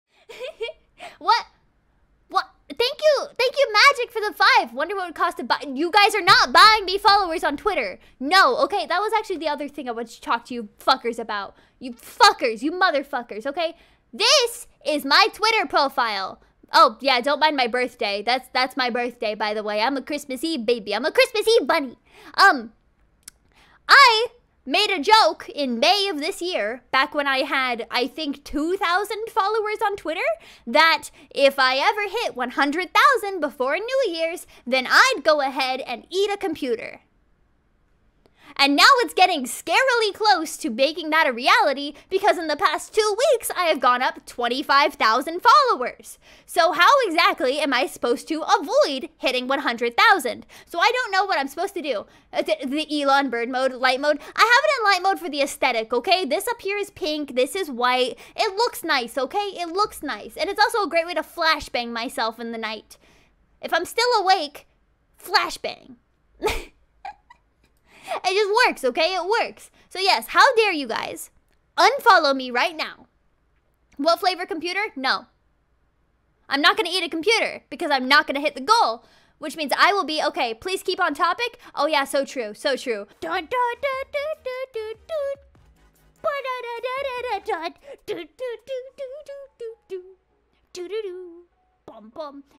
ATTENTION!!! Those who watch this video please DO THIS for our beloved Jim [Punkalopi Clip]
PLEASE follow Punkalopi's Twitter so that she will eat computer!

This video is made to support her and to follow her Twitter not for making her to monch a real computer. And I'm sure none of us expect her to eat real computer. She can just make a computer-shaped cake or put edible picture of computer if she really reach 100k before new year. Anything goes as long as it cause no harm for her.
And I always wanna say thank you for your support for her, and for finding this channel. I hope you enjoy it!

Thank you for attention guys! Remember this is just a joke, there's no forcing you guys. This is for fun only ^^

Heya! Thank you for the visit and for the watch!